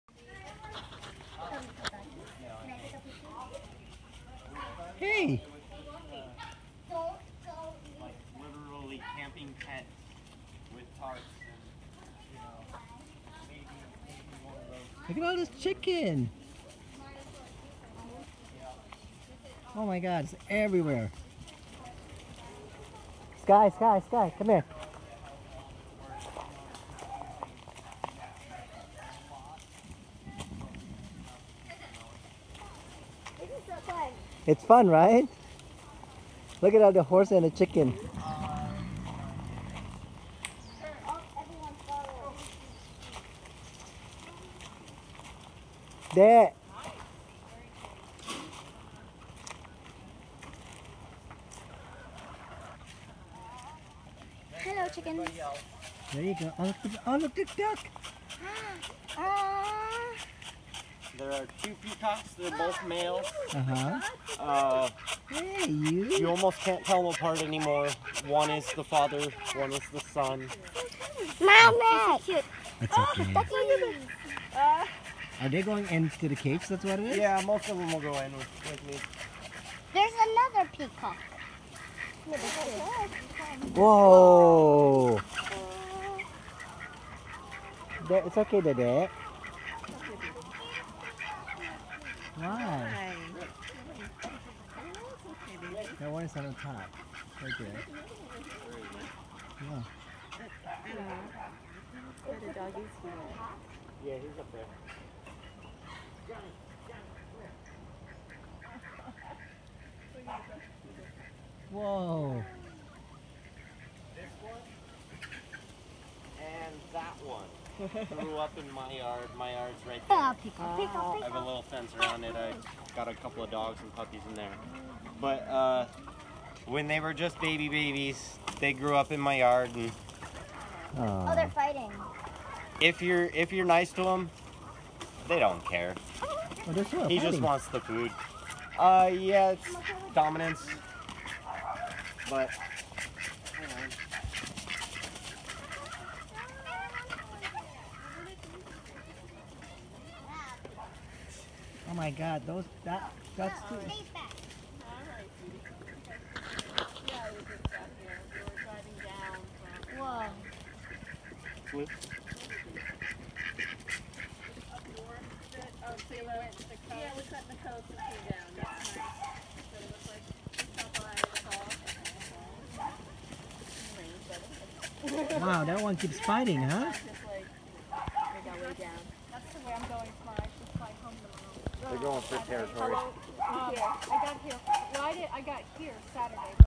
Feeding time at the farm :)
Big island, Hawaii.  Glamping with the rescued animals